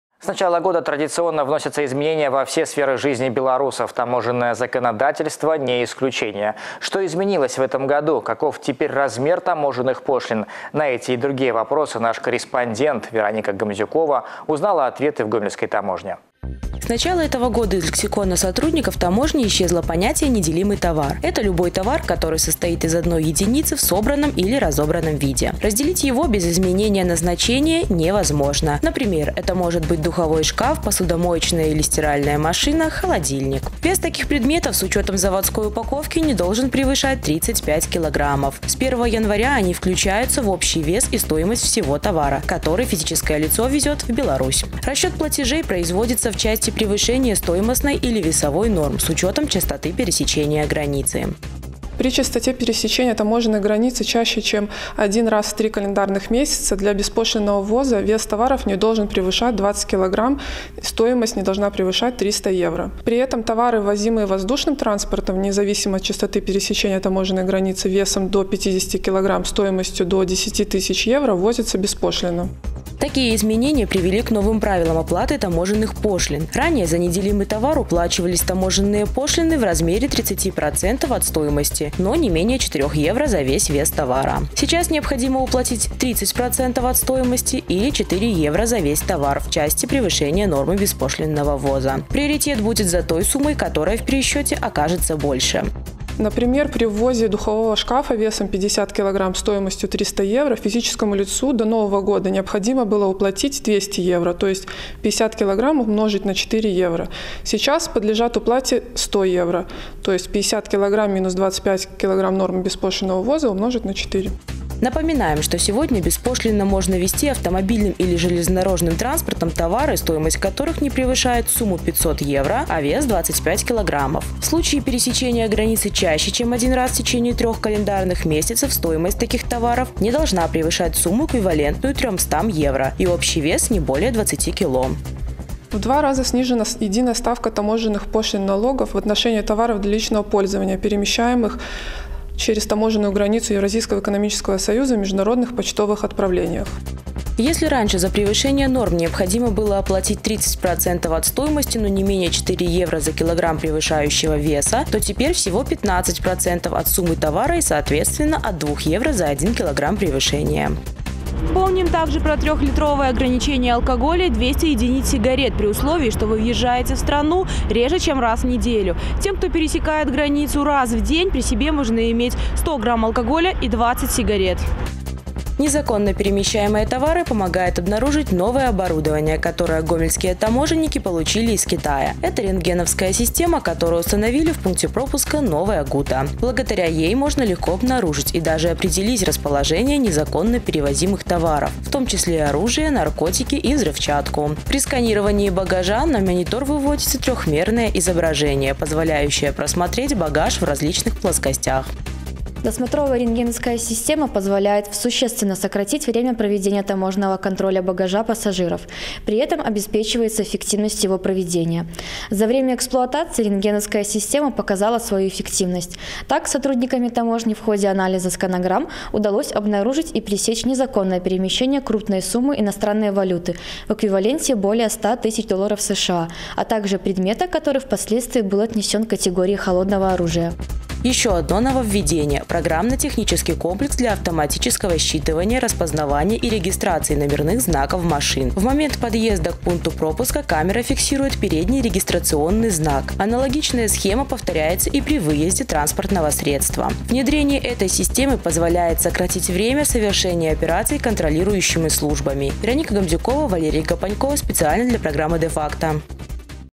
Репортаж: Изменения в Таможенном кодексе
С начала года традиционно вносятся изменения во все сферы жизни белорусов. Таможенное законодательство не исключение. Что изменилось в этом году? Каков теперь размер таможенных пошлин? На эти и другие вопросы наш корреспондент Вероника Гамзюкова узнала ответы в Гомельской таможне.